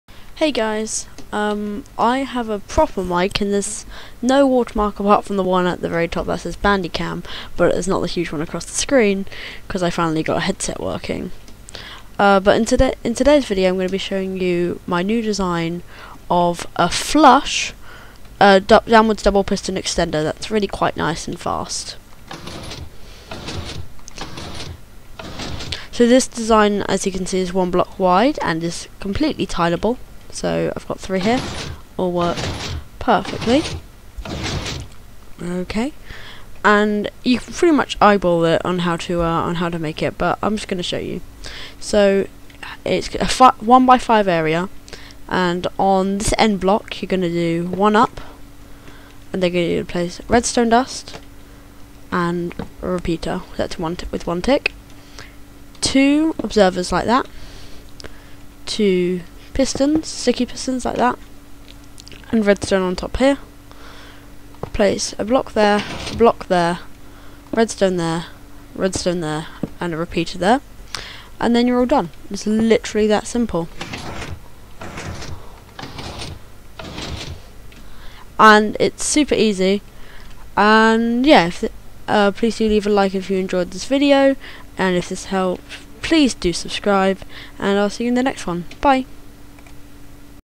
Simple Flush & Tileable Downwards Double Piston Extender | Turtorial
In this video, I showcase my new design of a downwards double piston extender. It is flush and tileable and extremely simple.